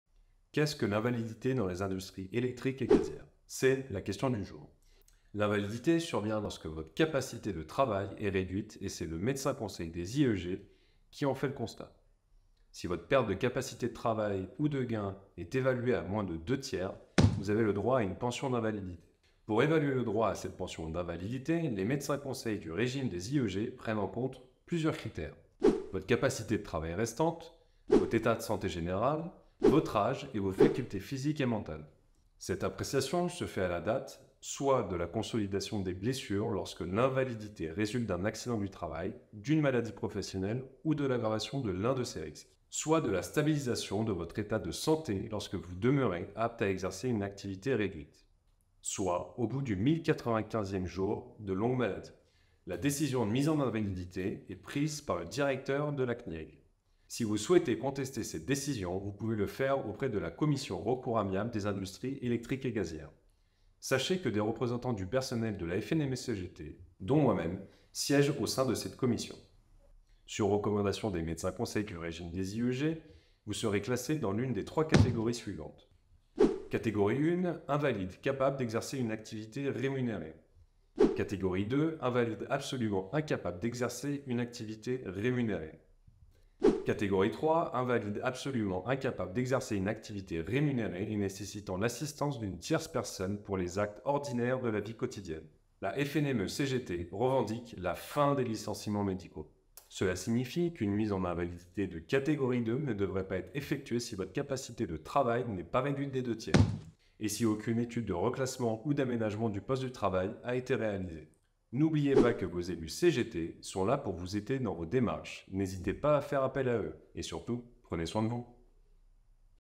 Invalidité dans les industries éléctriques et gazières
Qu'est-ce que l'invalidité dans les IEG ?
Découvrez les critères et vos droits.

Votre santé compte !
Si vous avez des questions, consultez vos élus CGT.